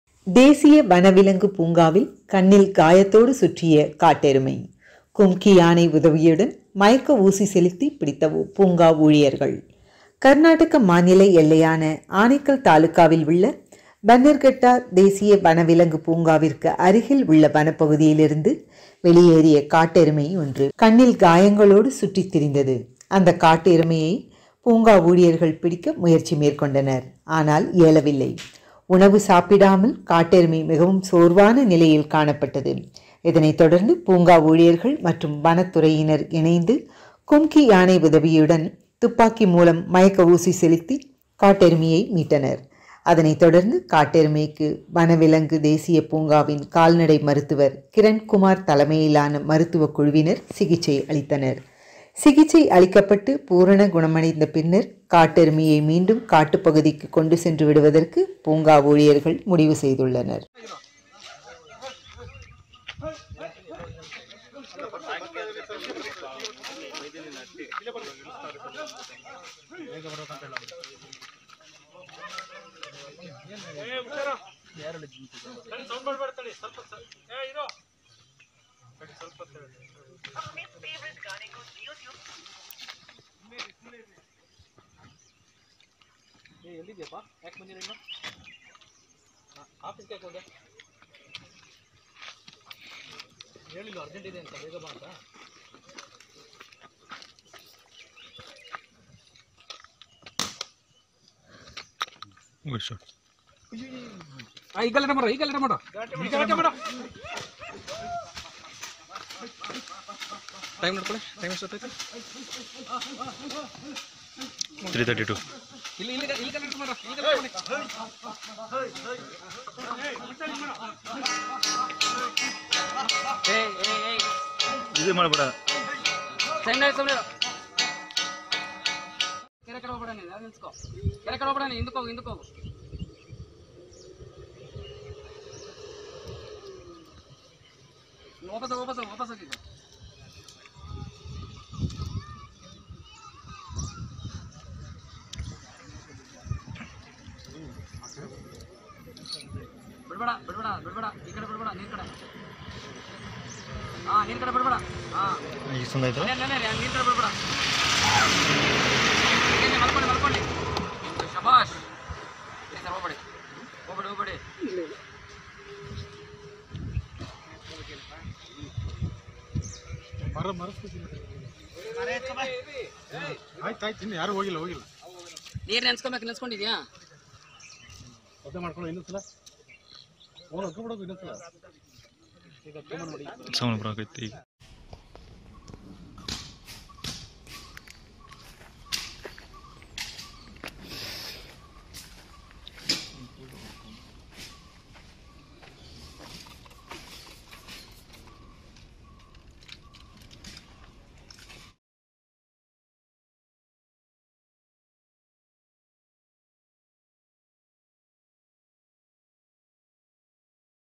தேசிய வனவிலங்கு பூங்காவில் காயத்தோடு சுற்றிய காட்டு எருமை மயக்க ஊசி செலுத்தி பிடித்த பூங்காஊழியர்கள்
தேசிய வனவிலங்கு  பூங்காவில் கண்ணில் காயத்தோடு சுற்றிய காட்டு எருமை : கும்கு யானை உதவியுடன், மயக்க ஊசி செலுத்தி பிடித்த பூங்கா ஊழியர்கள்

கர்நாடக மாநில எல்லையான ஆனெக்கல் தாலுகாவில் உள்ள பன்னார் கட்டா தேசிய வனவிலங்கு பூங்காவிற்கு அருகில் உள்ள வனப்பகுதியில் இருந்து வெளியேறிய காட்டு எருமை ஒன்று  கண்ணில் காயங்களோடு சுற்றித்திரிந்தது. அந்த காட்டு எருமையை பூங்கா ஊழியர்கள் பிடிக்க முயற்சி மேற்கொண்டனர். ஆனால் முடியவில்லை,

உணவு சாப்பிடாமல் காட்டெருமை மிகவும் சோர்வான நிலையில் காணப்பட்டது.
அதனைத்தொடர்ந்து பூங்கா ஊழியர்கள்  மற்றும் வனத்துறையினர் இணைந்து கும்கி யானை உதவியுடன், துப்பாக்கி மூலம் மயக்க ஊசி செலுத்தி காட்டெருமையை பிடித்து மீட்டனர். அதனைத் தொடர்ந்து அந்த காட்டு எருமைக்கு வனவிலங்கு தேசிய பூங்காவின் கால்நடை மருத்துவர் கிரண் குமார் தலைமையிலான மருத்துவ குழுவினர் சிகிச்சை அளித்தனர்.

சிகிச்சை அளிக்கப்பட்டு பூரண குணமடைந்த பின்னர் காட்டு எருமையை மீண்டும் காட்டு பகுதிக்கு கொண்டு சென்று விடுவதற்கு பூங்கா ஊழியர்கள் முடிவு செய்துள்ளனர்.